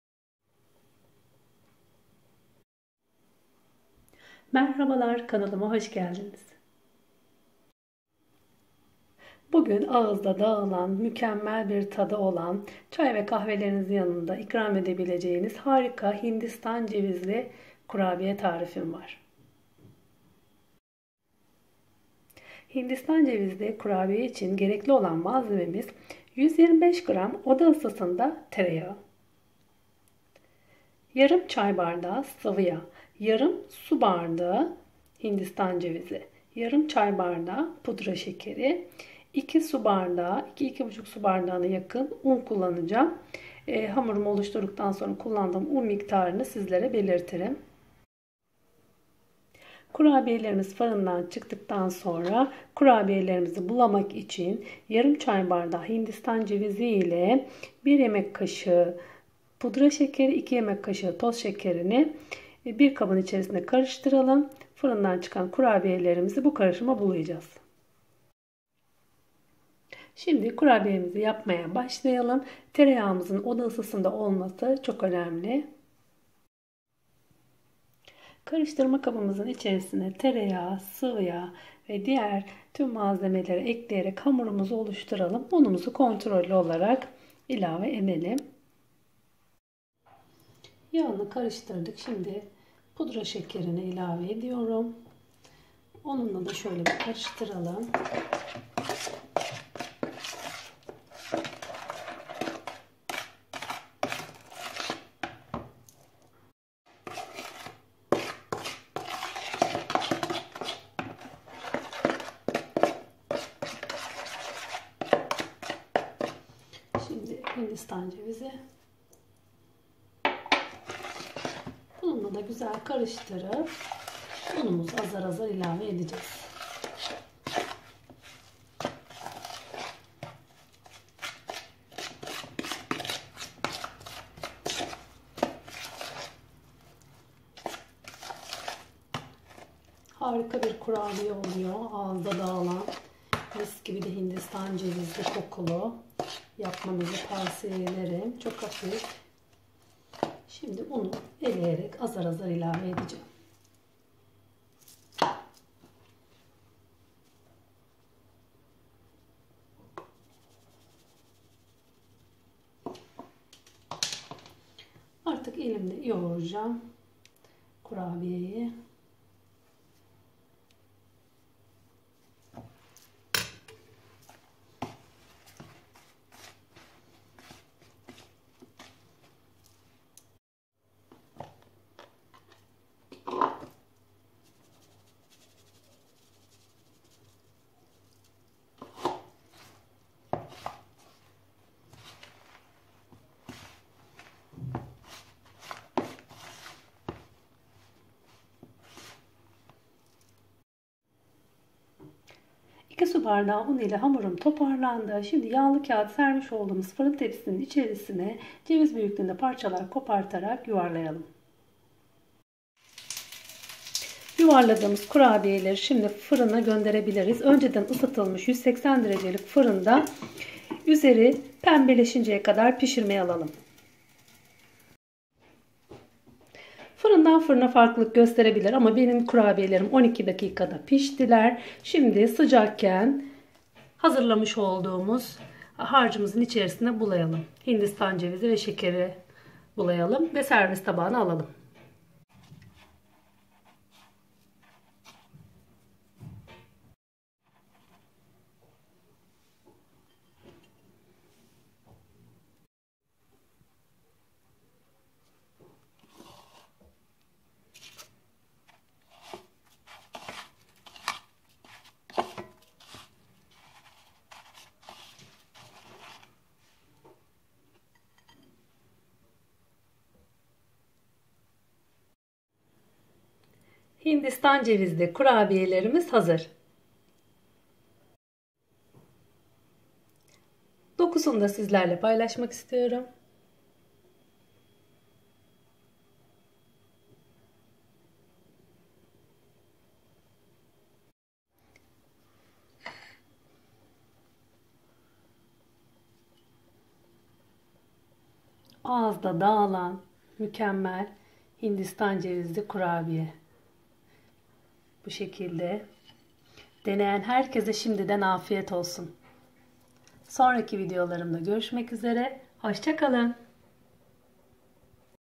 Hindistan cevizli kurabiye
125gr.tereyagi, yarım çay bardağı  sıvıya, yarım çay bardağı pudra şekeri, yarım su bardağı Hindistan cevizi, 2su bardağı un.bulamak için 2yemek kaşığı pudra şekeri 1yemek kaşığı toz şeker yarım çay bardağı h.cevizi